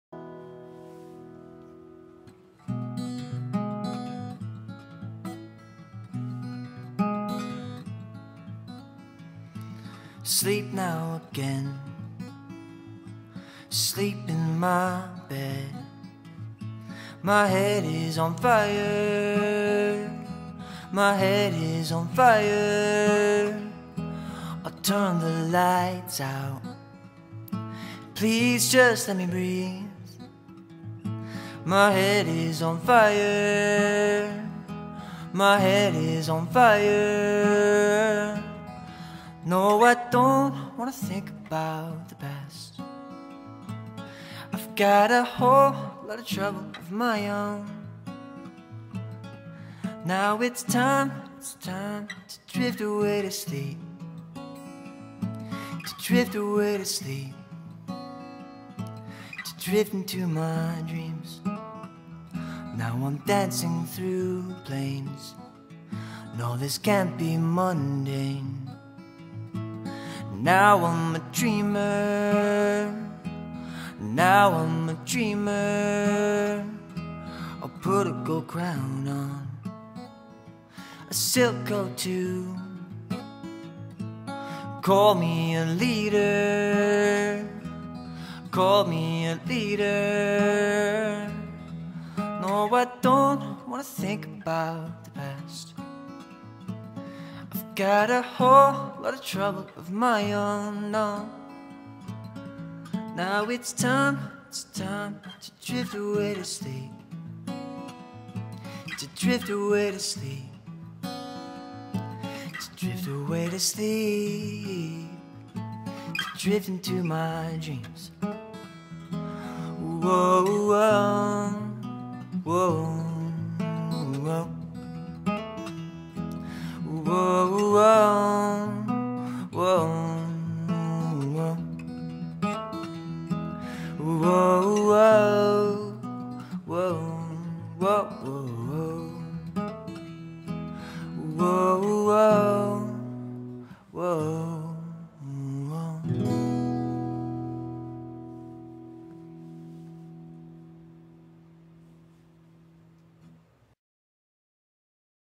Michael Alleckna Live at Tractorgrease
This is the 6th year of 89.5 The Drive's Music Business 101 program. Thx for being a big part of the local music scene.

All videos filmed at Tractorgrease Studio